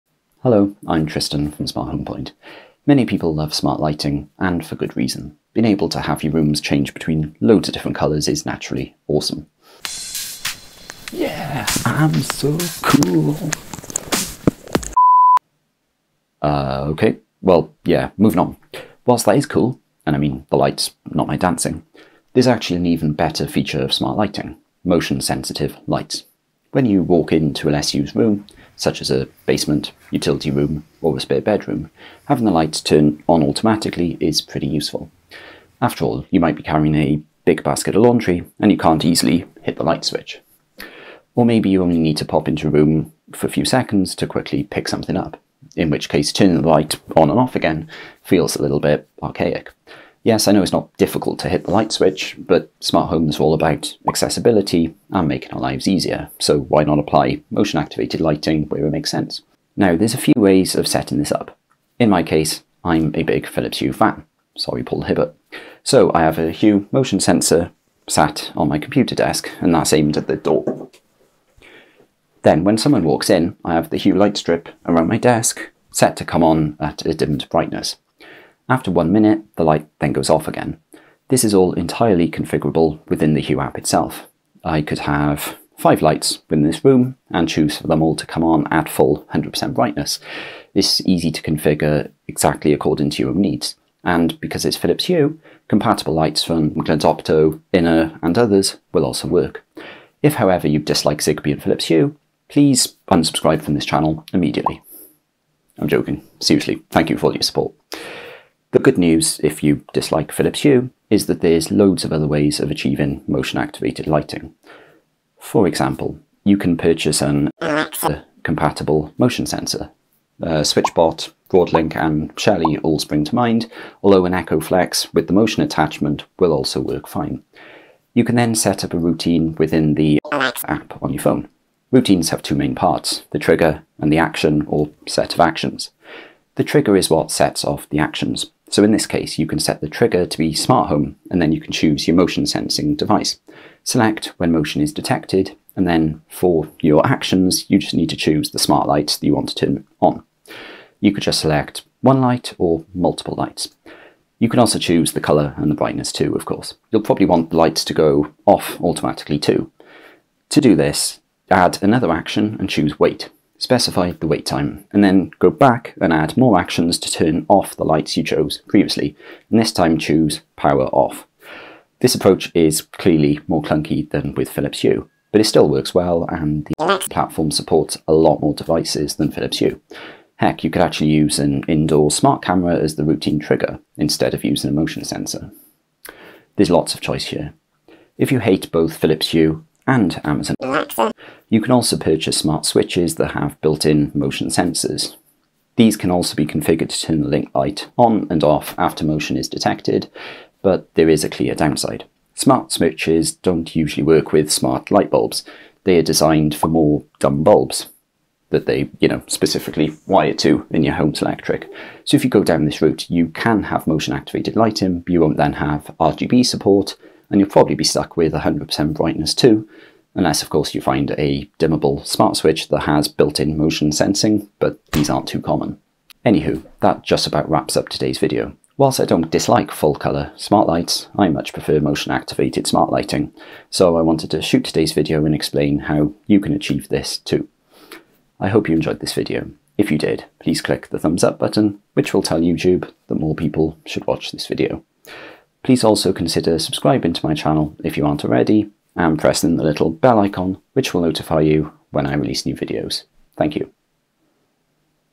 Why I Love This 'Hidden' Smart Lighting Feature (& How To Set It Up)
When people think of smart lighting, they usually think of the obvious: full color (RGB) bulbs. Setting rooms to bright green, or dancing to strobe lighting effects.

But I actually prefer a less discussed smart lighting feature: motion sensitive lighting. This is simple to set up, and it feels like something out of sci-fi.

I discuss how to achieve this simple-but-powerful functionality in today's video.

0:00 Intro
1:02 Philips Hue approach
1:48 Alexa approach
3:10 Smart switches approach
3:46 Wrapping up

Or you can use a Lutron Caseta sensor such as the PD-OSENS-WH or PD-VSENS-WH